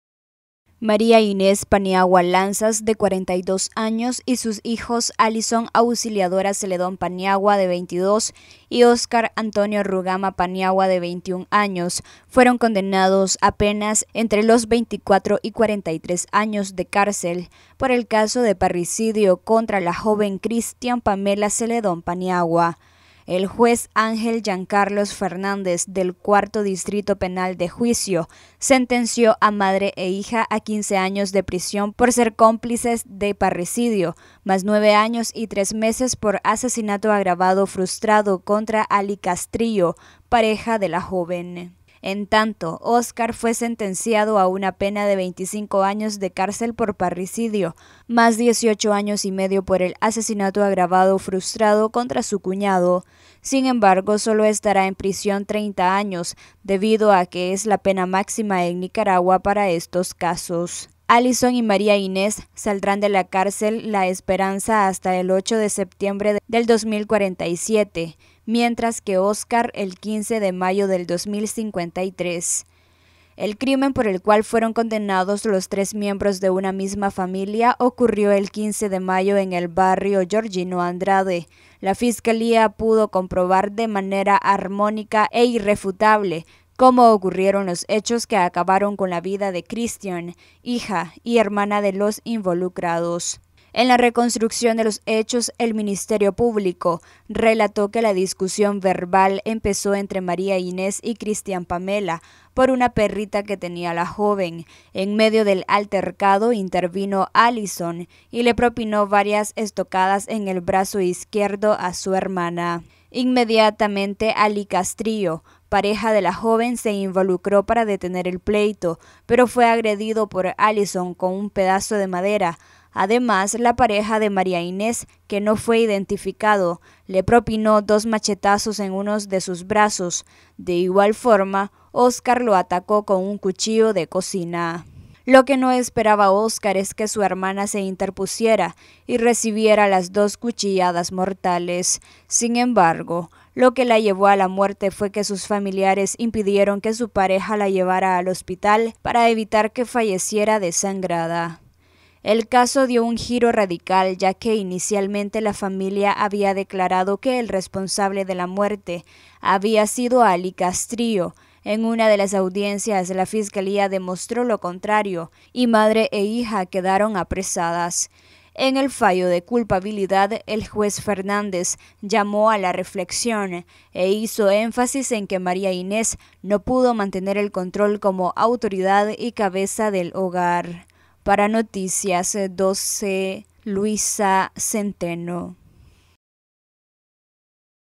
Noticias12Nicaragua Madre e hijos sentenciados a larga condena por parricidio
Madre e hijos sentenciados a larga condena por parricidio